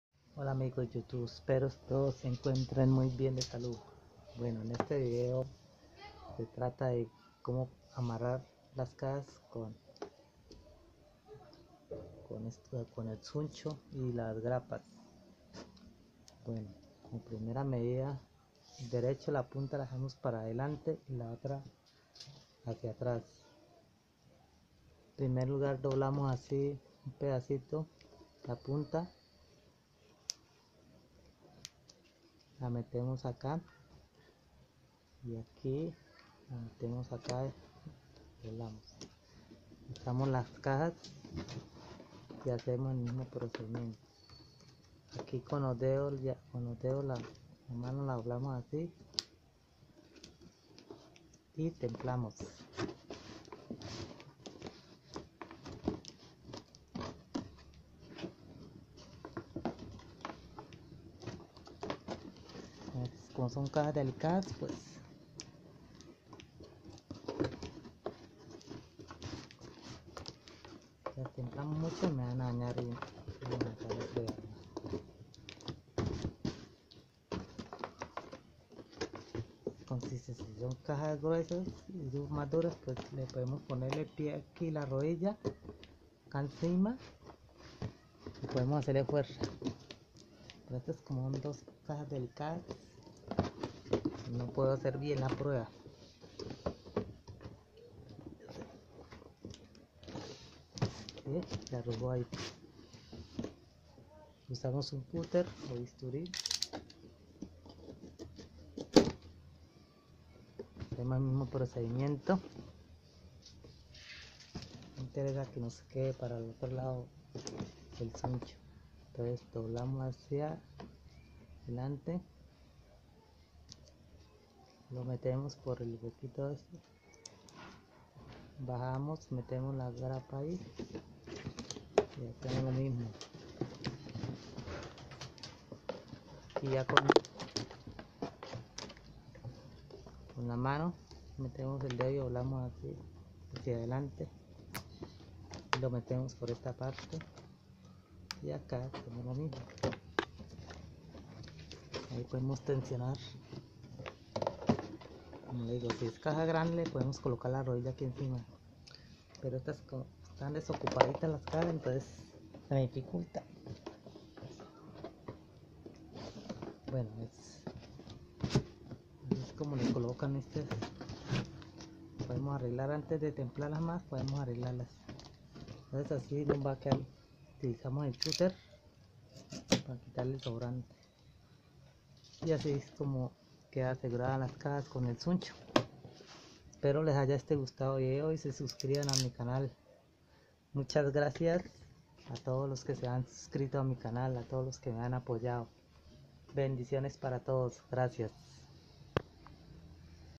COMO AMARRAR CAJAS CON ZUNCHO🚀
Reforzar cajas con amarres llamado zuncho con el cual nos facilita la manipulacion del articulo para poderlo transportar con menos delicadeza y apilar a cierta altura y si hay derrumbe de carga no se dañe mucho la mercancia